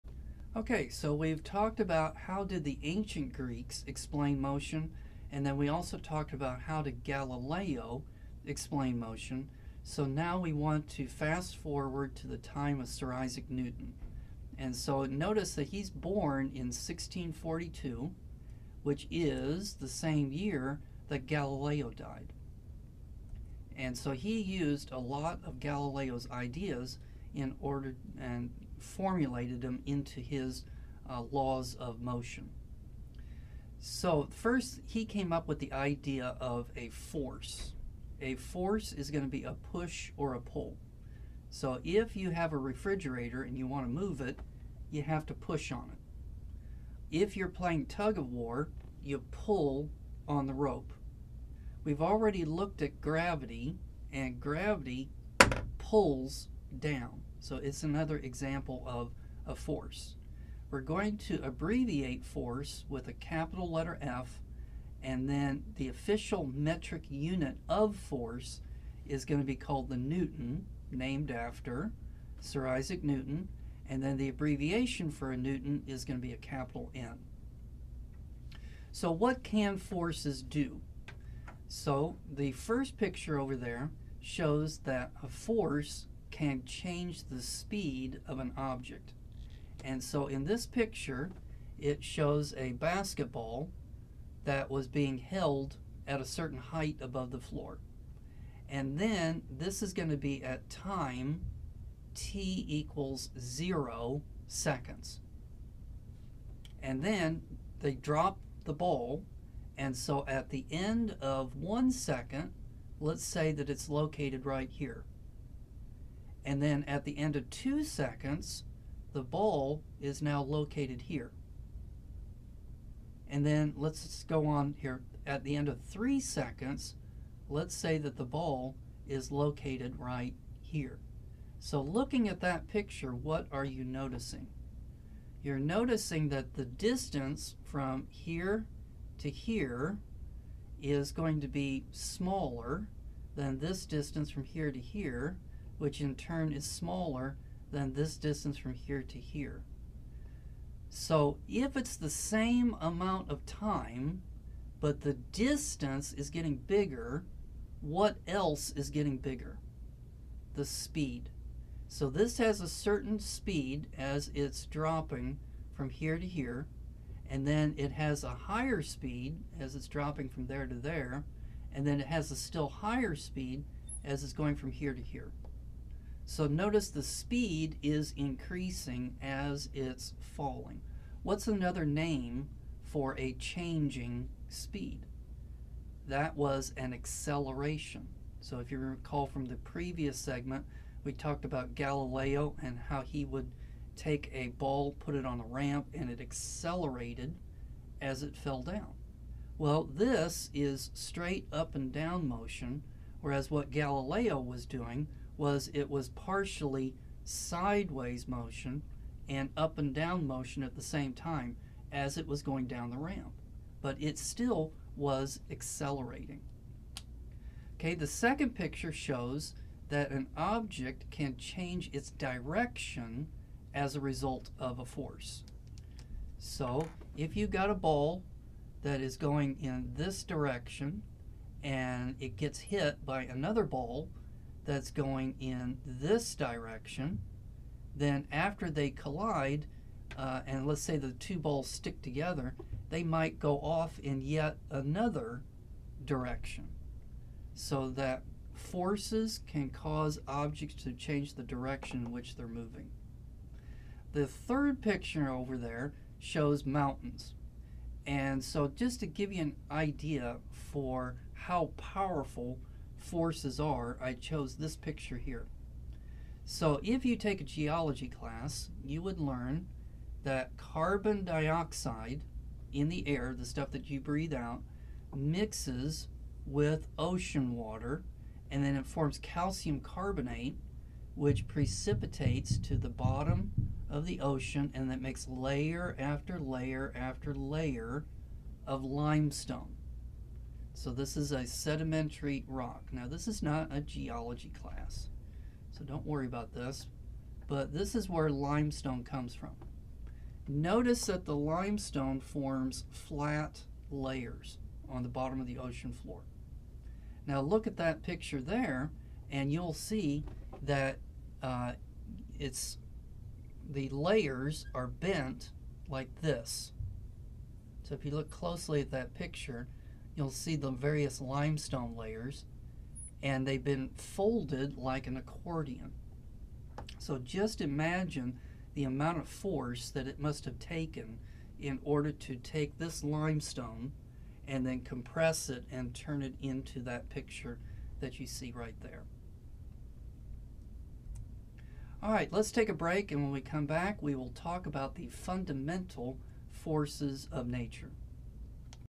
Phy 2 3 What Forces Are
In this segment about Newton's 1st Law of Motion you will learn what a force is and what it does.